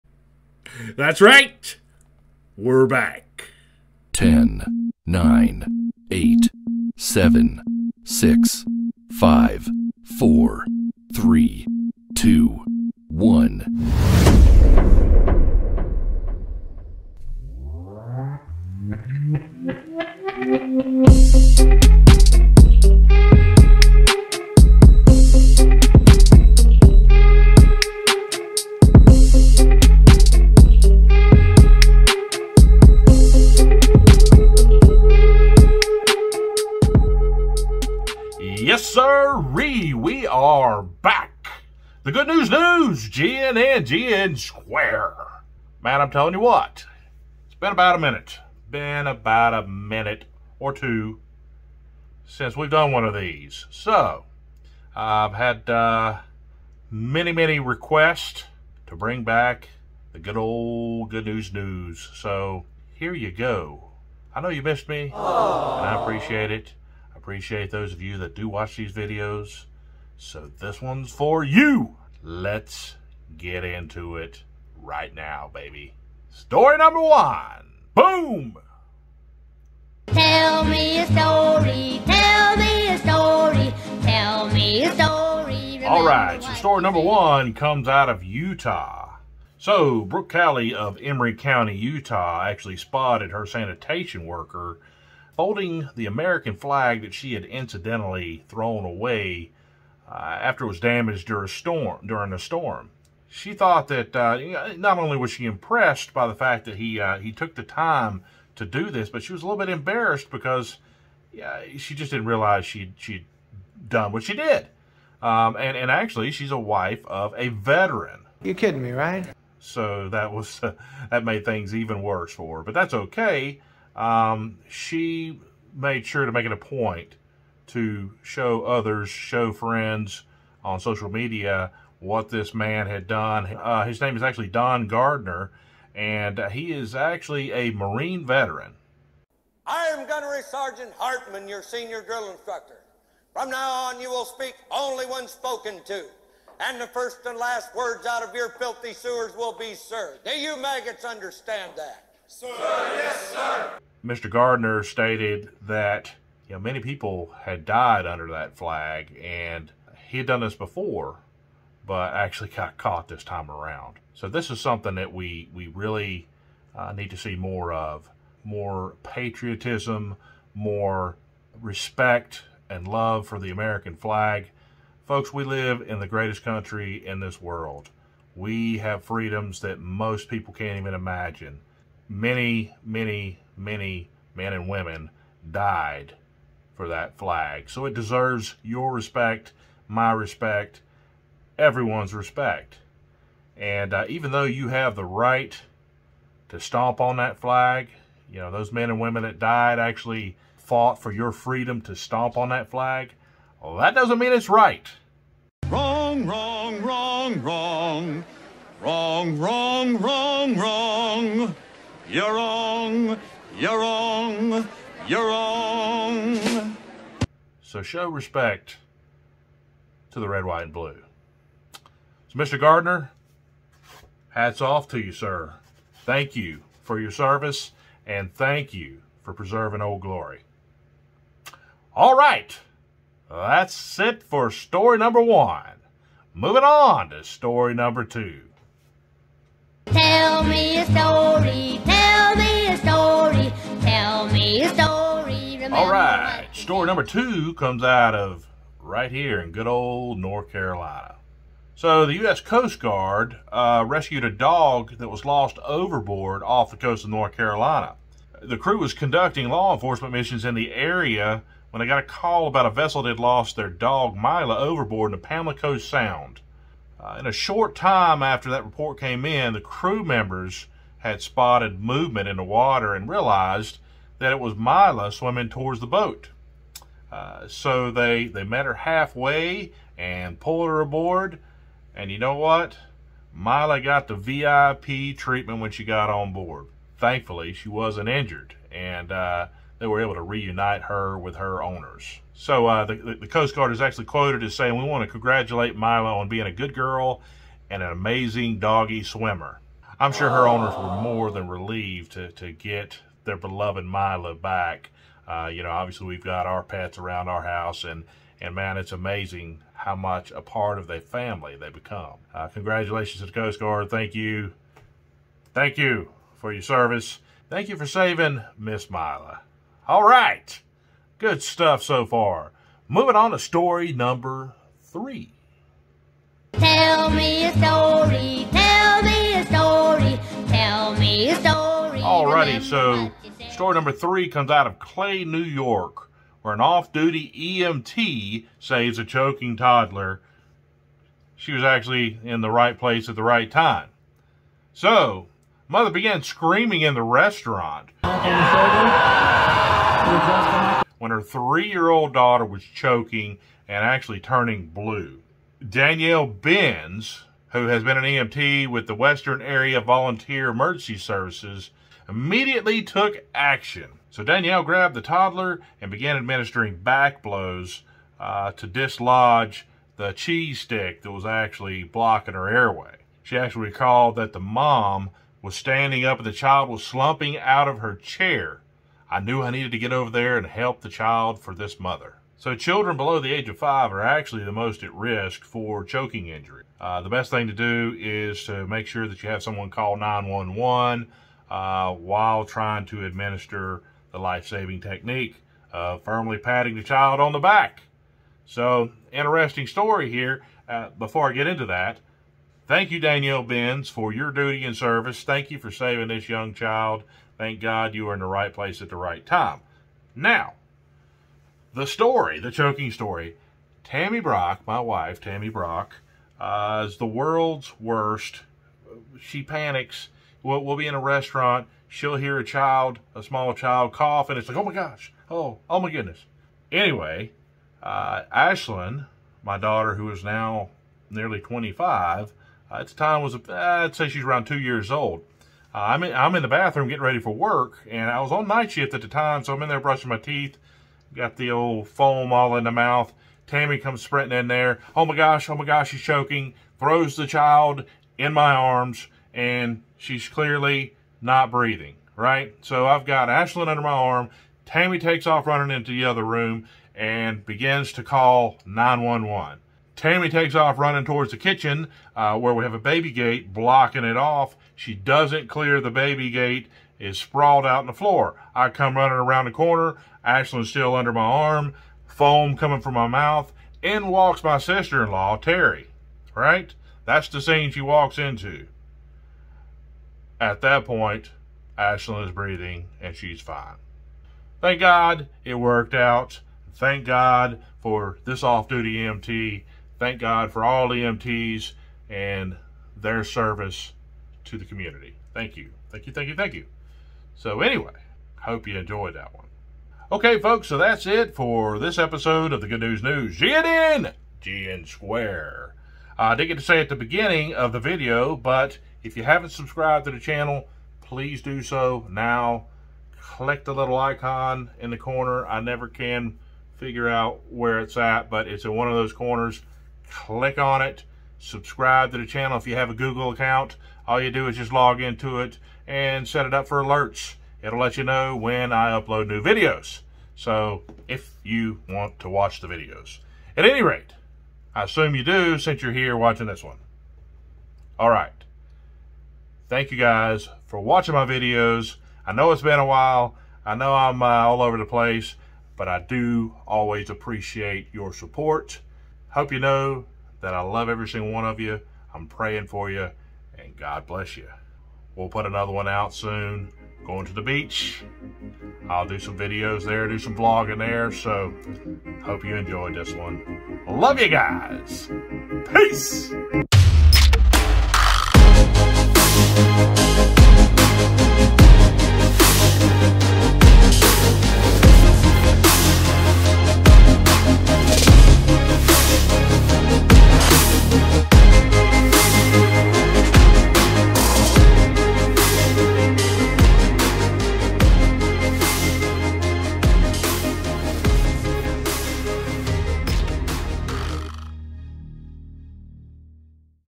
U S Coast Guard saves overboard dog in Pamlico Sound GN² EP15
Utah sanitation worker saves flag, U.S. Coast Guard saves overboard dog in Pamlico Sound, Off duty EMT saves choking toddler